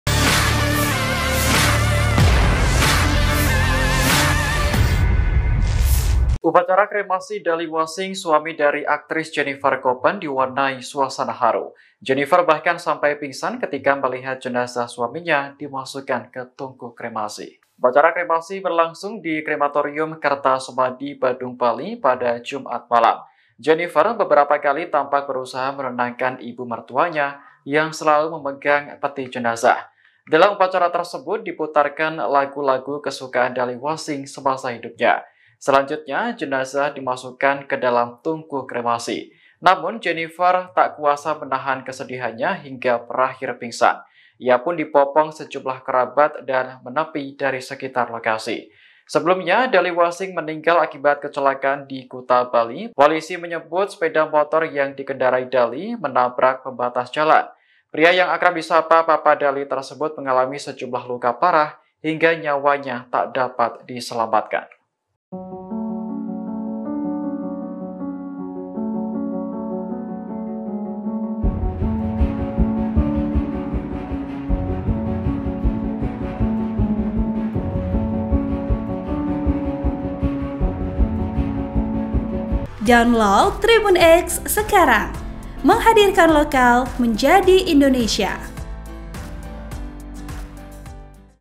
Jenifer coppen Pingsan Tak Kuat Tahan Kesedihan, Jenazah Dali Wassink Hendak Dikremasi
TRIBUN-TIMUR.COM - Upacara kremasi Dali Wassink, suami dari aktris Jennifer Coppen diwarnai suasana haru.

Jennifer bahkan sampai pingsan ketika melihat jenazah suaminya dimasukkan ke tungku kremasi.

Upacara kremasi berlangsung di Krematorium Kertha Semadi, Badung, Bali, pada Jumat (19/7/2024) malam.

Jennifer beberapa kali tampak berusaha menenangkan ibu mertuanya yang selalu memegang peti jenazah.

Dalam upacara tersebut, diputarkan lagu-lagu kesukaan Dali Wassink semasa hidupnya.

Selanjutnya jenazah dimasukkan ke dalam tungku kremasi.

Namun, Jennifer tak kuasa menahan kesedihannya hingga berakhir pingsan.

Ia pun dibopong sejumlah kerabat dan menepi dari sekitar lokasi.

Sebelumnya, Dali Wassink meninggal akibat kecelakaan di Kuta, Bali pada Kamis (18/7/2024) dini hari.

Polisi menyebut, sepeda motor yang dikendarai Dali menabrak pembatas jalan.

Pria yang akrab disapa Papa Dali tersebut mengalami sejumlah luka parah hingga nyawanya tak dapat diselamatkan.

(TRIBUN-TIMUR.COM)